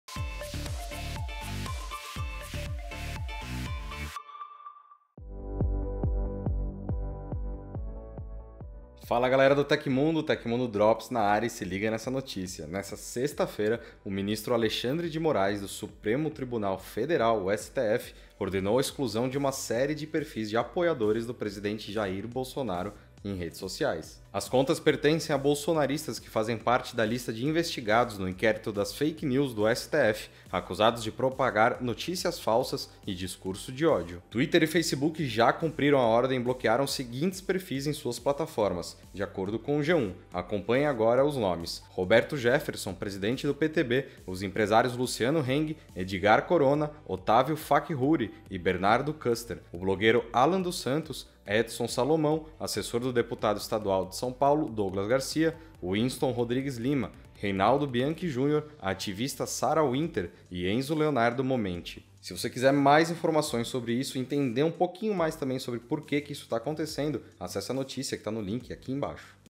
Twitter e Facebook bloqueiam contas de bolsonaristas – TecMundo Drops
Twitter e Facebook bloquearam contas de bolsonaristas após ordem judicial do STF.

🤑Se liga nas ofertas que encontramos pra vocês!!!🤑
Amazon Prime (30 dias grátis!)
Music Unlimited (3 meses grátis!)
É só até amanhã!!!***

[Tipo: Drops]

CRÉDITOS
Adaptação: Felipe Payão
Apresentação: Felipe Payão
Edição de vídeo: Rafael Nogoceke
Motion Graphics: Gustavo Silva
Arte da thumb: Fernando Perazzoli